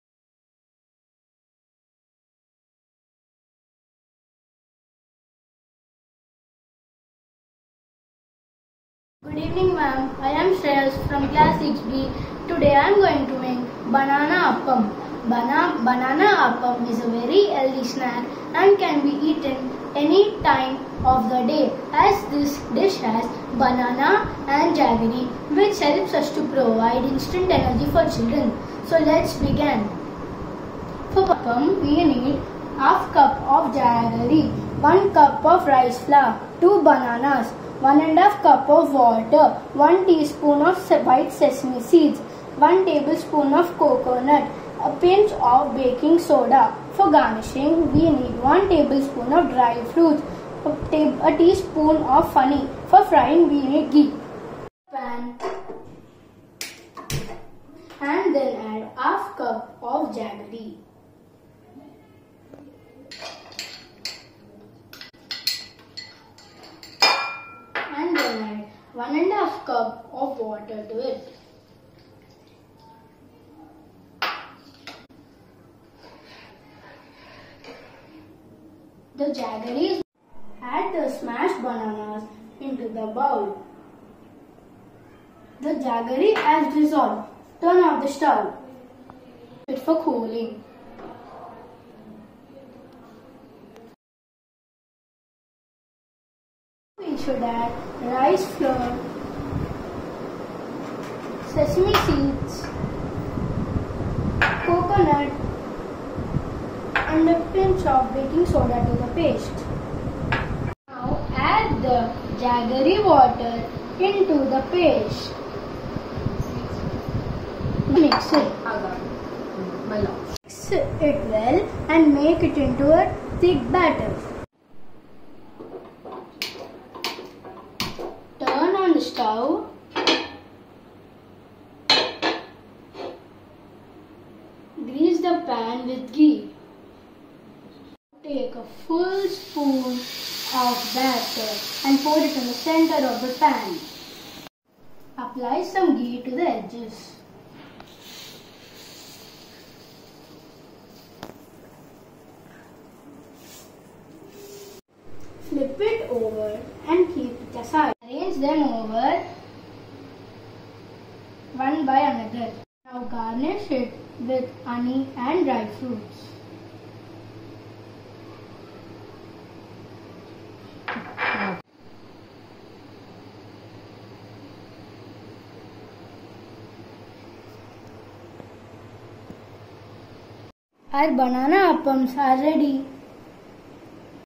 Flavours | Cooking Competition
We appreciate the participation of T.P Shreyas of Class 6 in the Juniors category Cooking Competition.